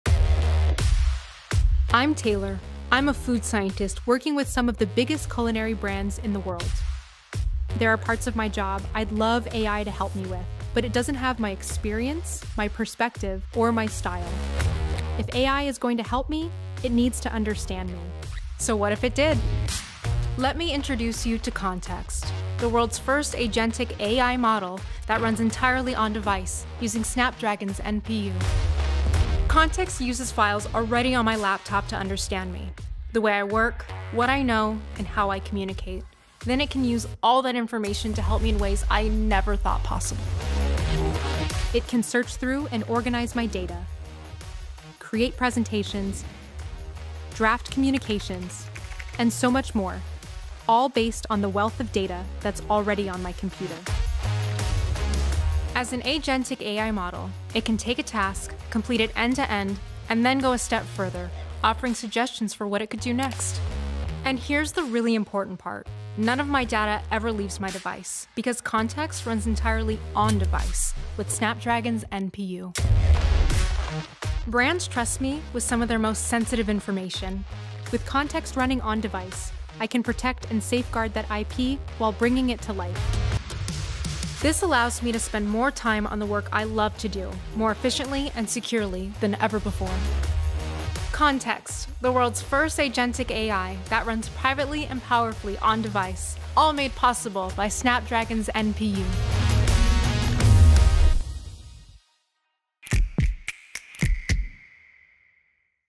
The Future of AI is On-Device | Meet Context, Powered by Snapdragon X Series
AI that knows you—without knowing your data. Context is the world's first agentic AI model that runs entirely on-device, powered by Snapdragon X Series. Unlike cloud-based AI, Context operates securely on your laptop, using your existing files to understand your work style, knowledge, and communication methods. It can perform tasks end-to-end, anticipate your needs, and offer intelligent suggestions while keeping your sensitive data private.

With no information ever leaving your device, Context is a game-changer for professionals handling confidential data, especially in industries like food science, where intellectual property (IP) is essential. Trusted by brands and experts, it ensures that security and privacy remain uncompromised while delivering powerful AI-driven efficiency. Experience the future of on-device intelligence with Context.